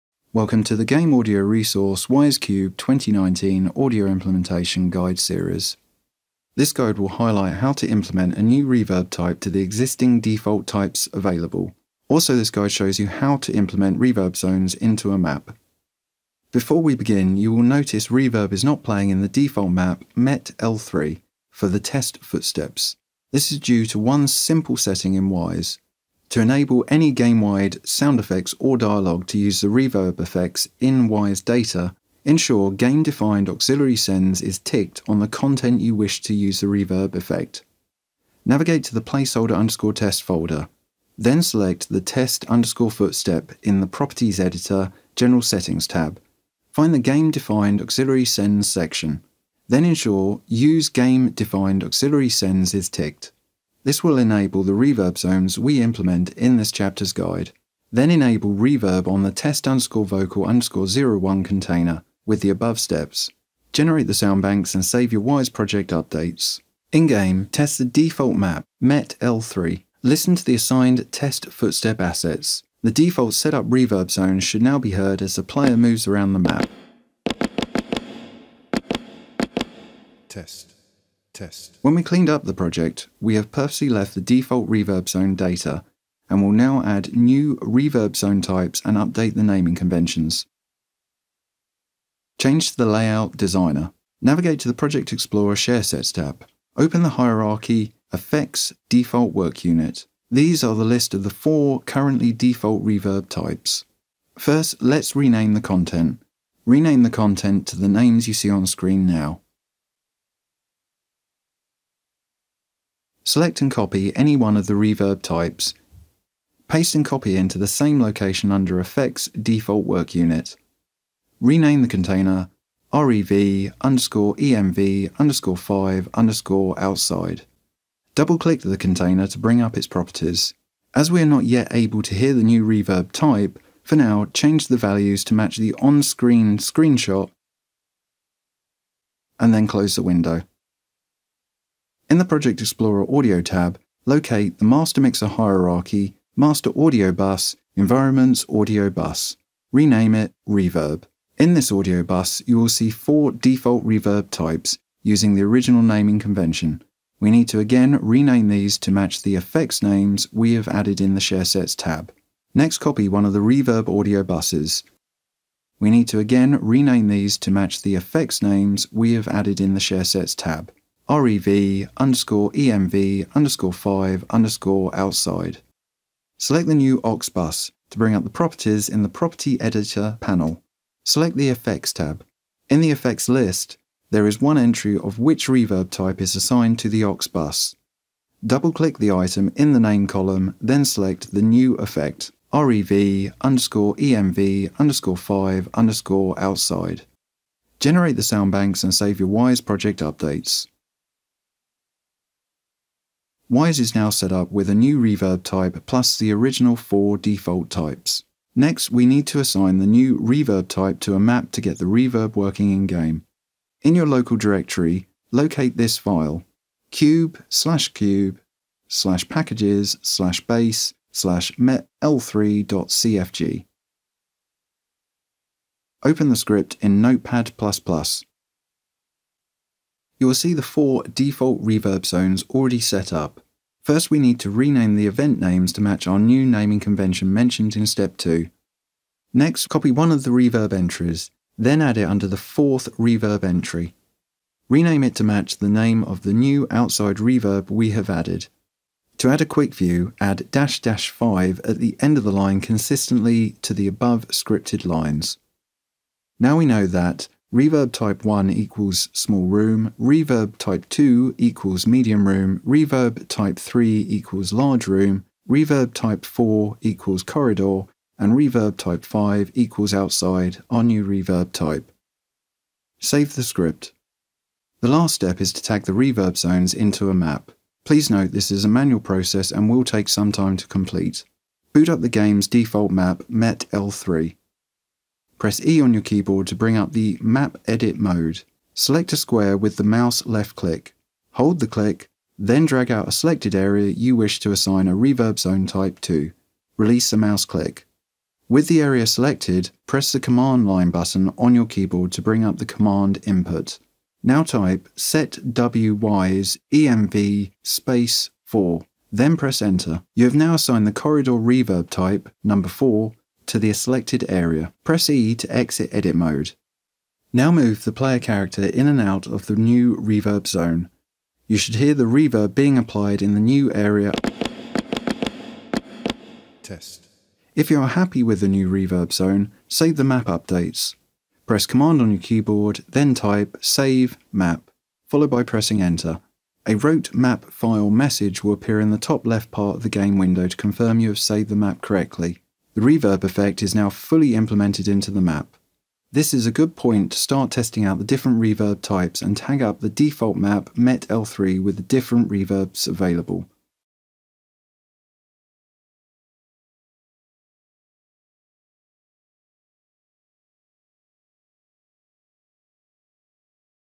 Wwise Cube 2019: Chapter 06 B - Environment: Reverb Zones
UPDATE 2019:
In this chapter step we look at how to tag unique environment reverb zones to any map. Also, we highlight how to add new extra reverb zone types that we can also use when tagging each map.